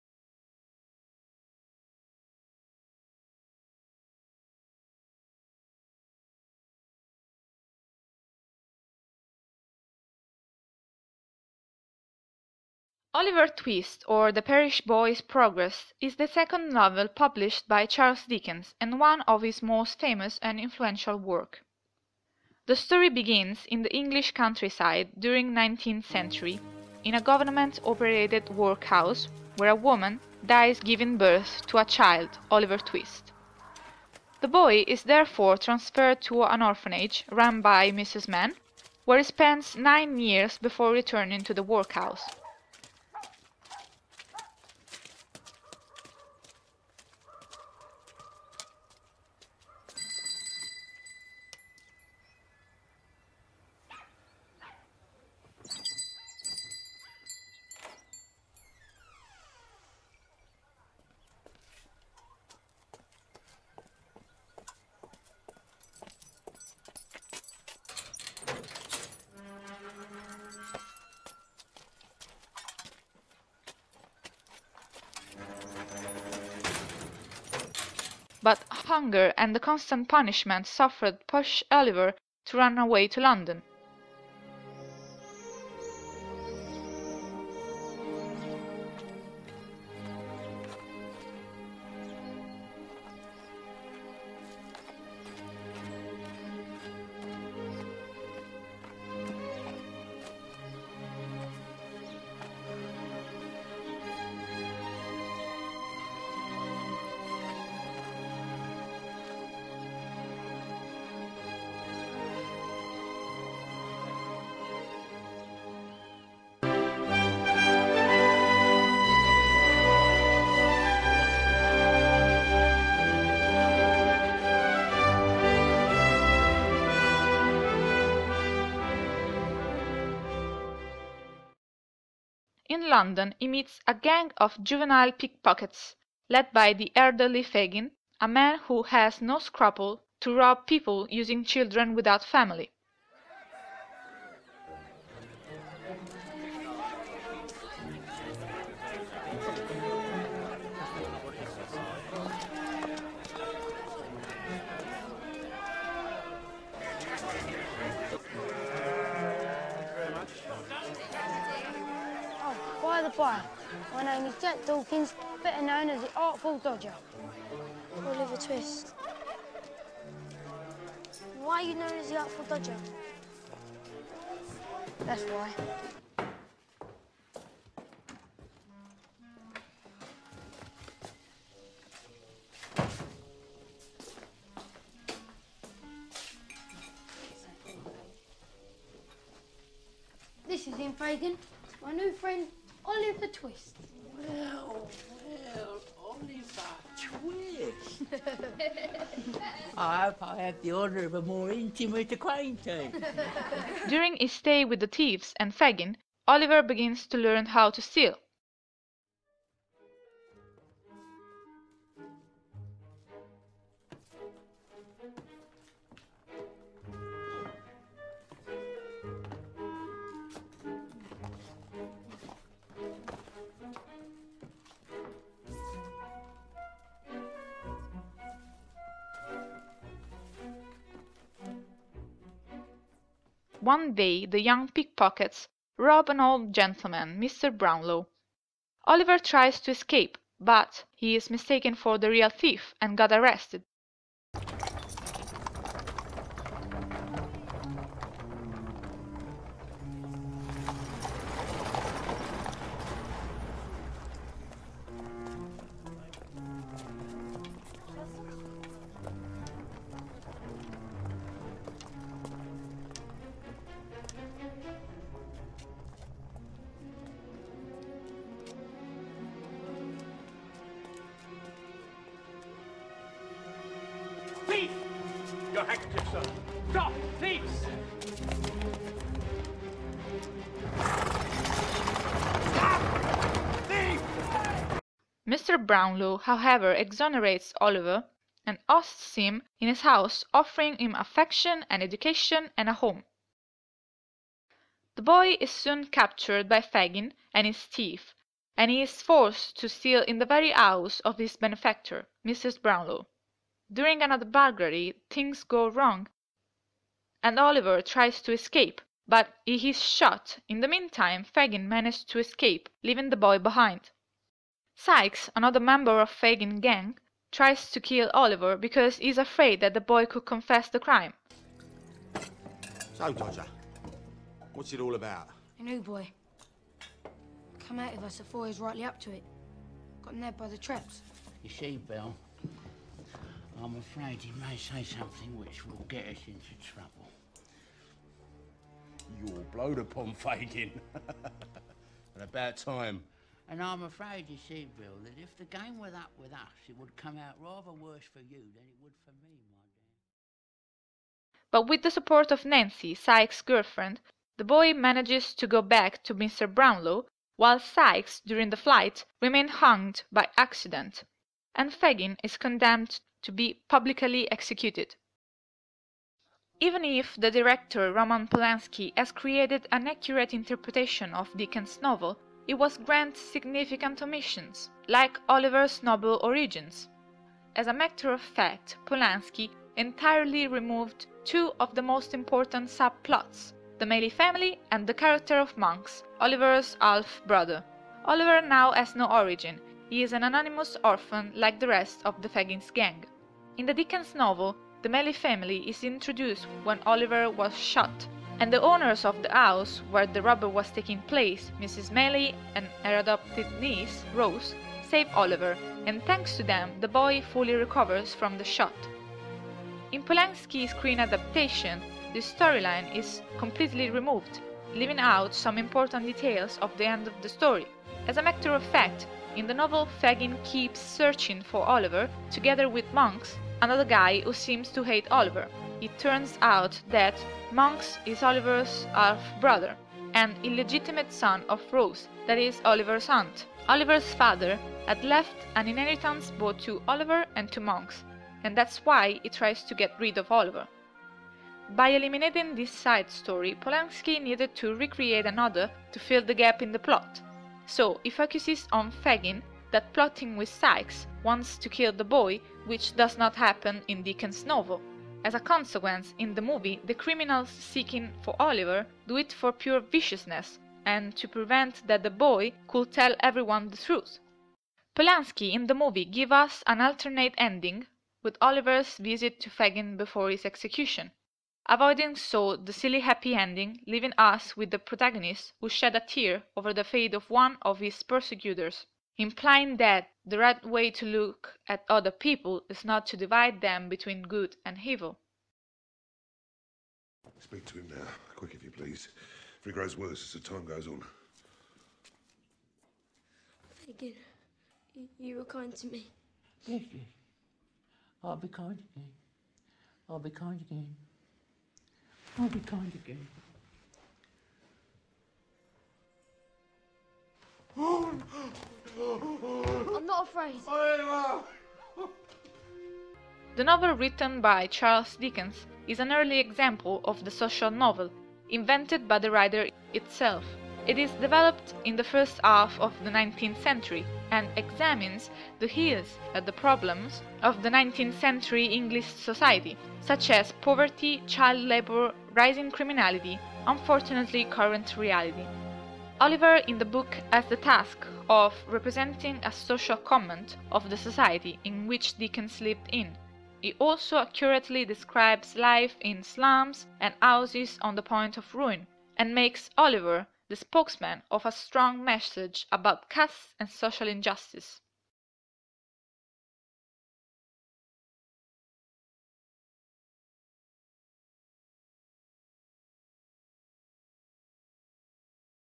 Maria Claudia Pelini - Oliver Twist ENG
Il seguente video è stato realizzato per l'esame finale di ''Literary Adaptation and Creative Documentaries'' (a.a. 2014/2015) presso la SSML Gregorio VII di Roma.
Un ringraziamento speciale a: la Prof.ssa Adriana Bisirri, nonché direttrice e presidente di Astrambiente e di AUPIU ed il Prof. Alfredo Rocca.